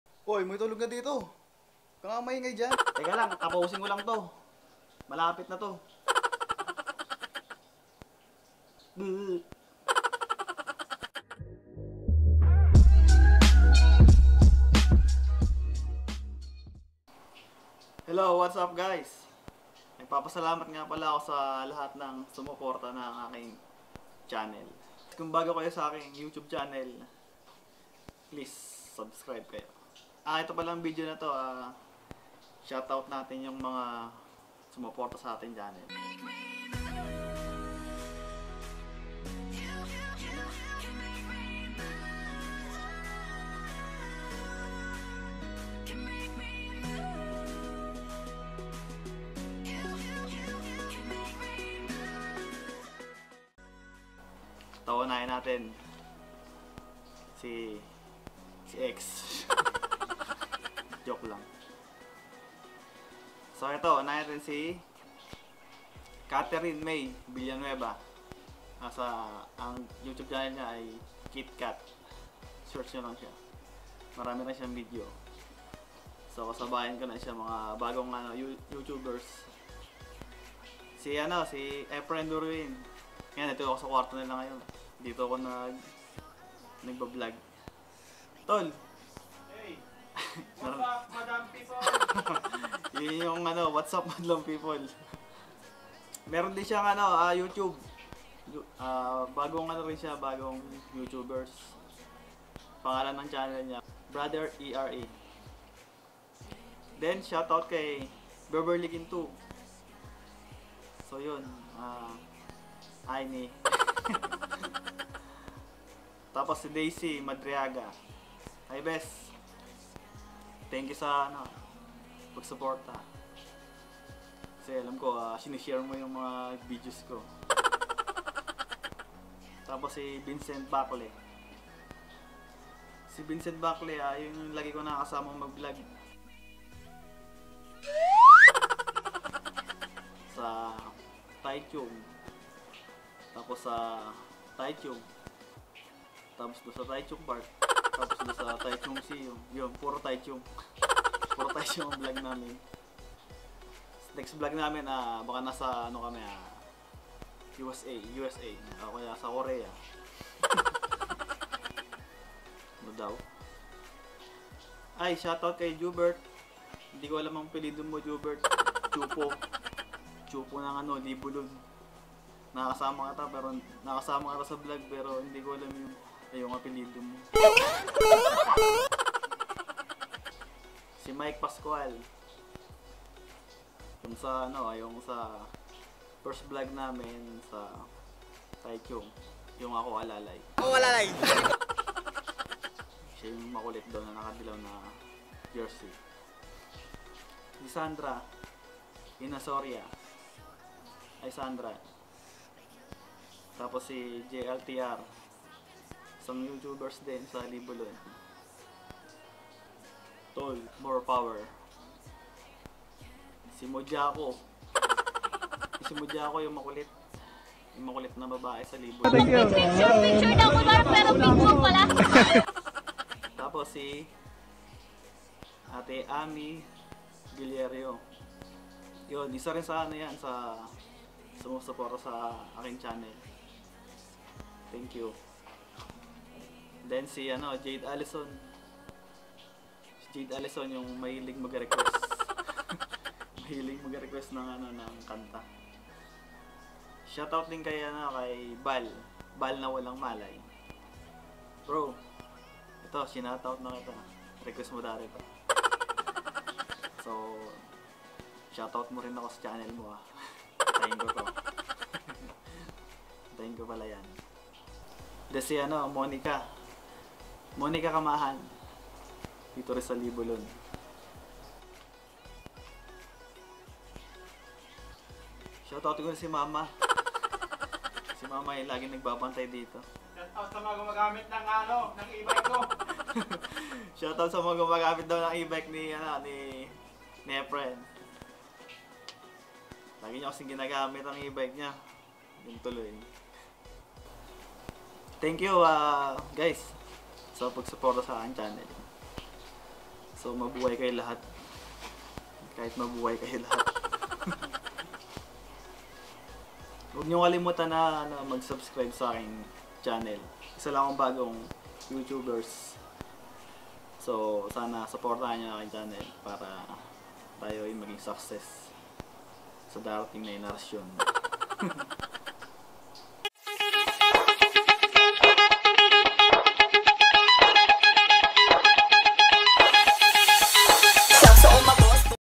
SHOUTOUT VIDEO|Para sa mga friends naten.
Gumawa ako ng Shoutout Video para sa mga friends naten na laging nanunuod ng aking channel more thank you hehe..

Name:

Catherine villanueva
Efren duruin
Baberlie Guintu
Vincent Bacle
Jubert no apelyido
Mike Pascual
Sandra Inasoria
JLTR
Mojako-mhe lhianne
Ame Guillero
Jhade Allyson
Vhal Na walang Malay
Monica Camahalan